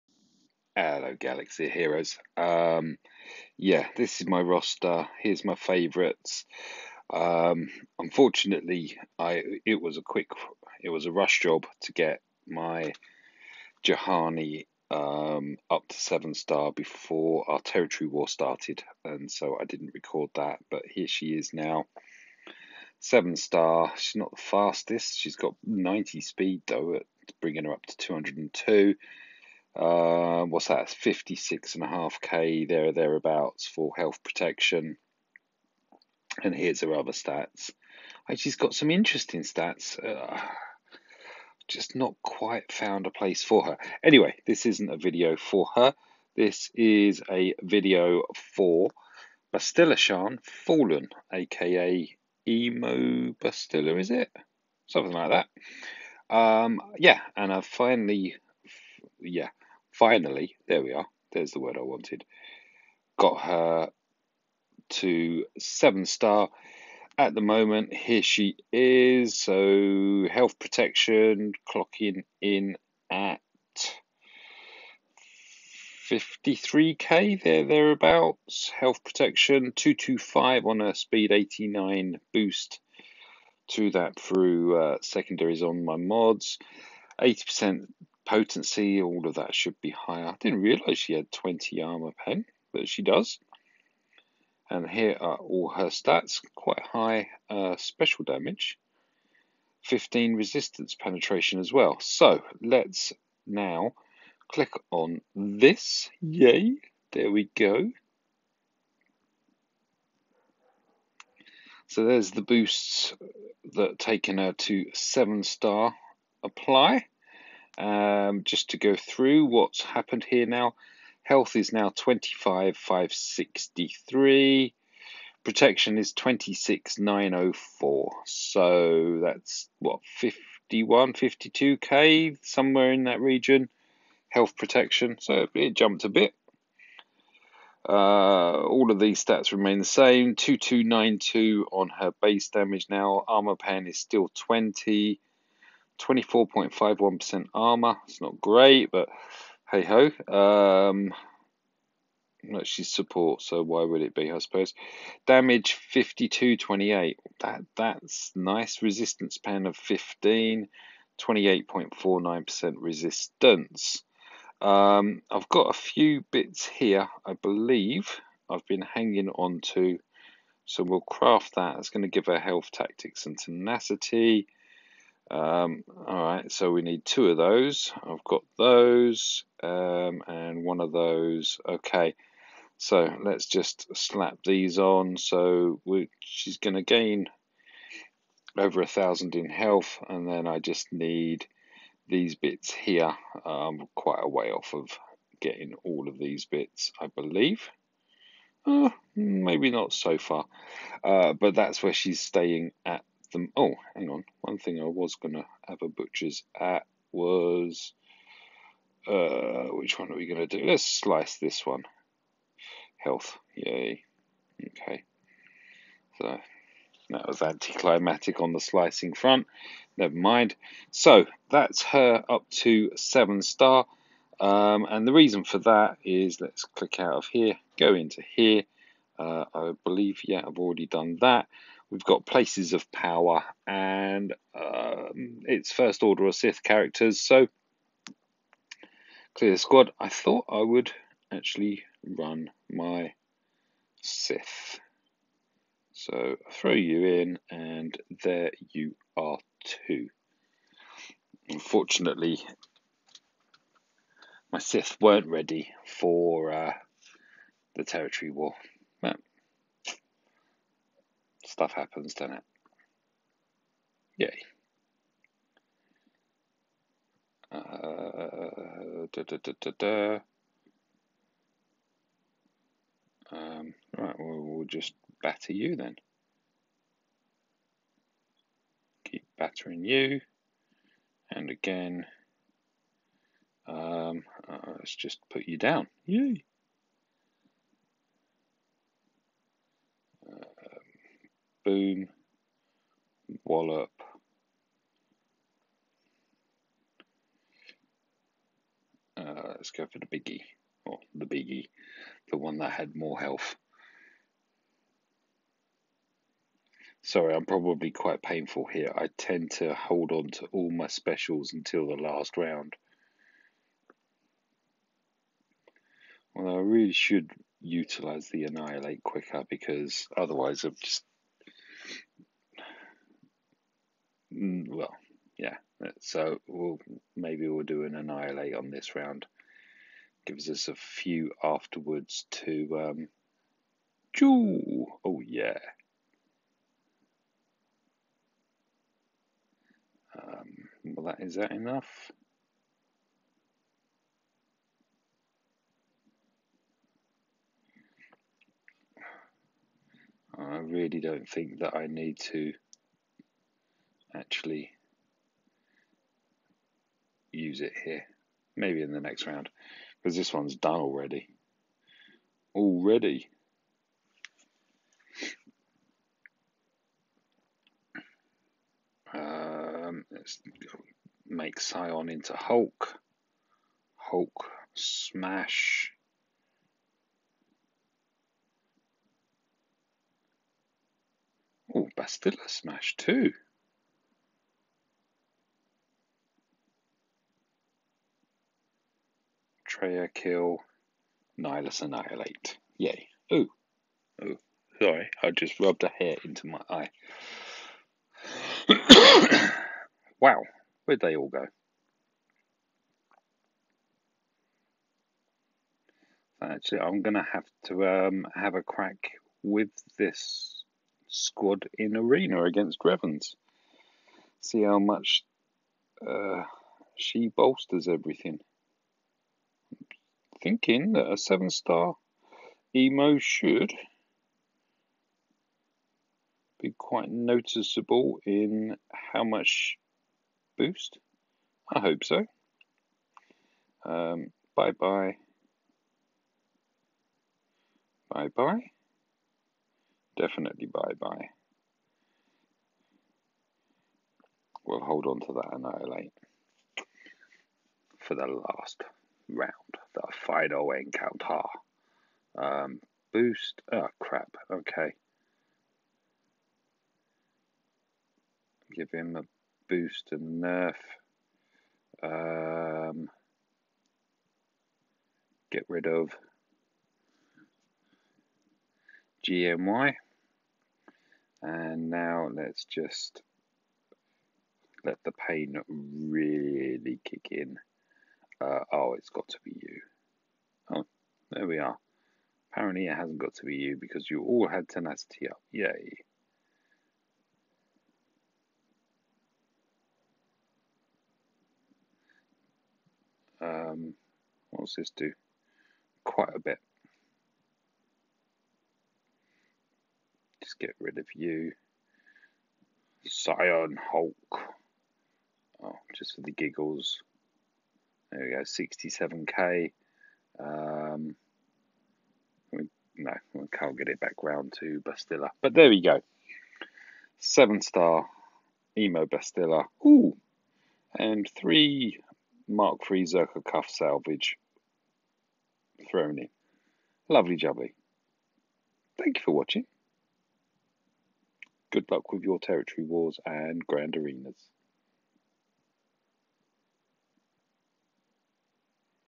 Gearing up BSF and Places of Power:Bonus Tier
Yay another toon farmed and I should be ready now for Triple Zeta Gamorrean Guard 😂